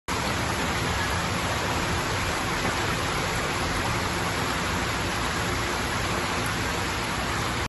Rotura de una tubería de agua en la zona del Cambrón de Toledo
Rotura de una tubería de agua durante las obras en la zona del Cambrón de Toledo.